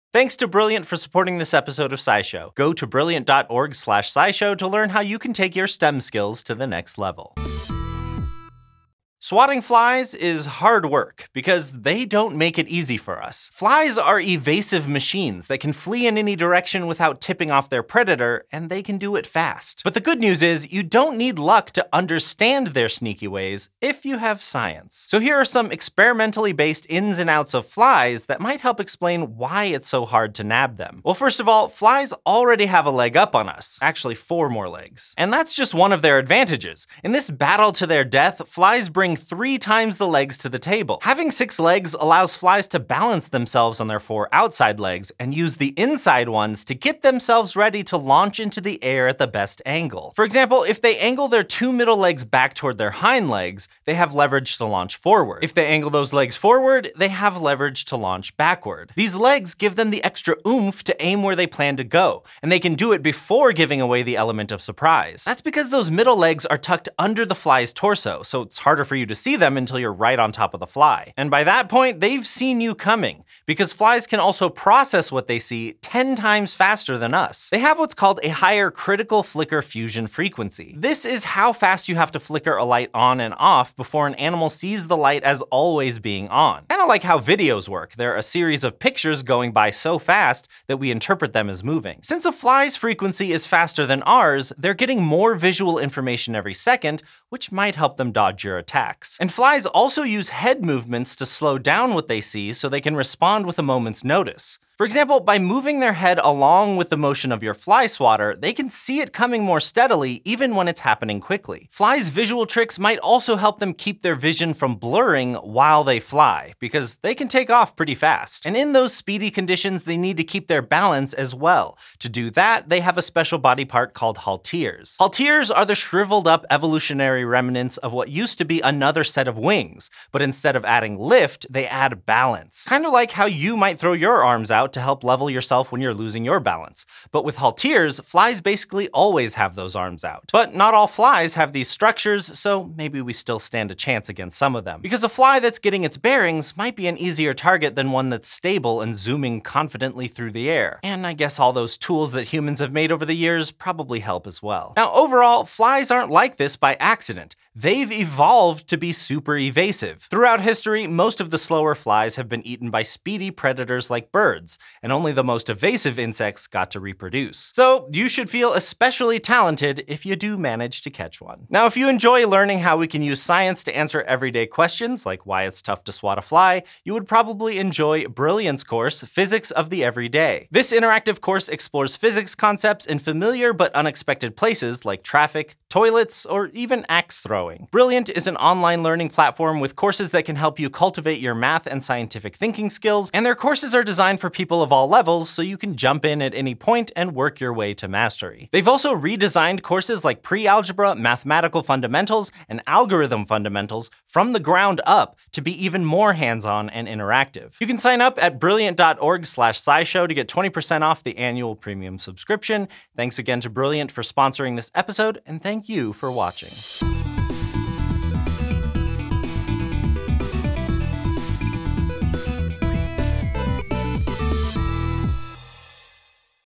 Why Is It so Hard to Swat a Fly?
Flies are evasive buzzing machines that make it nearly impossible to swat. Luckily, science has some explanation to help you predict their next move.